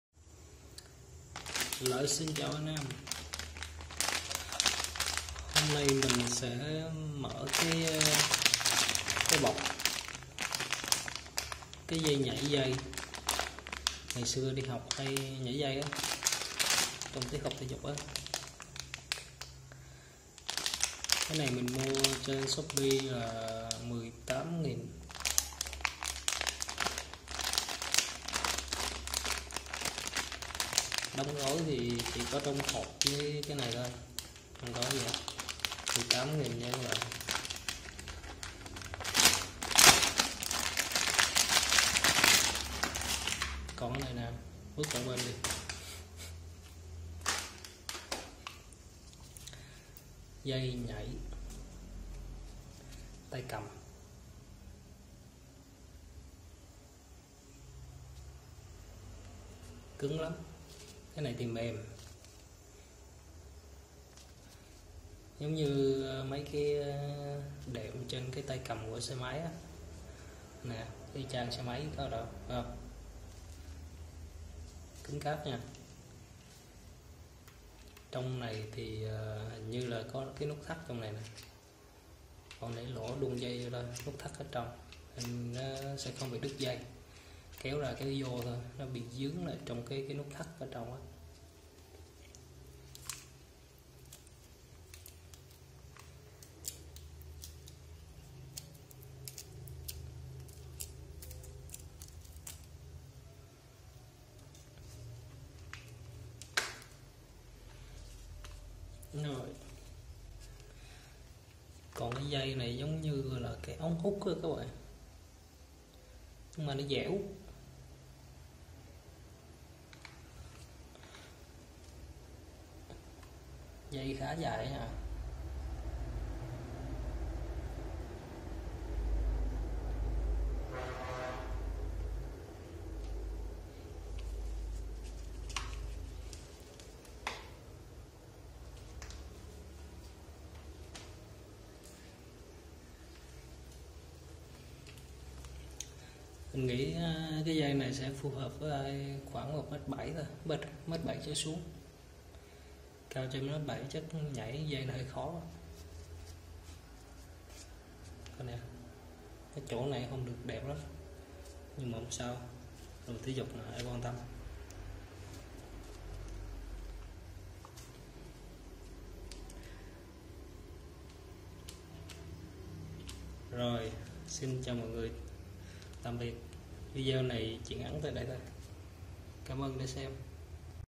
Dây nhảy thể dục giá rẻ 18K mua từ shopee có tốt không đây?
Dây nhảy thể dục giá rẻ 18K mua từ shopee có tốt không đây? dây nhảy tập thể dục để rèn luyện sức khỏe này mình thấy khá mềm và nhẹ. Một dụng cụ rèn luyện sức khỏe nhỏ gọn dễ sử dụng và giá rẻ.